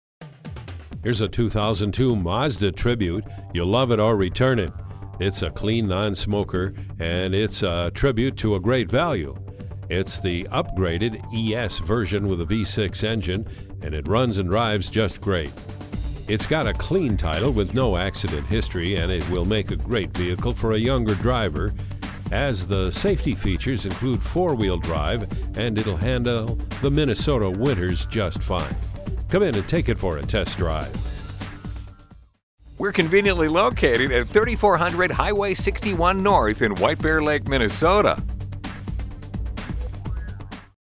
2002 Mazda Tribute St-Paul White-Bear-Lake, MN 68687A SOLD
Year: 2002
Make: Mazda
Model: Tribute
Trim: 3.0L Auto ES 4WD
Engine: 3.0 L V6 Cyl.
Transmission: Automatic
Color: Light Parchment Gold
Interior: Beige Leather
Mileage: 141460
Stock #: 68687A
4-wheel-drive, leather, tow hitch - this clean non-smoker Mazda is a Tribute (ha ha!) to great value! It's the upgraded ES version with the V6, and it runs and drives great. The AC is cold, the cruise and power options all work, and it's also got an upgraded sound system with MP3 jack and USB audio as well as HD radio! It's got a clean title with NO accident history and would make a great vehicle for a younger driver, as the safety features and 4-wheel-drive will handle the Minnesota winters just fine. Clean SUVs in this price range do not last long in our inventory, so please call ahead for availability at - talk to you soon!

Address:
3400 Highway 61 N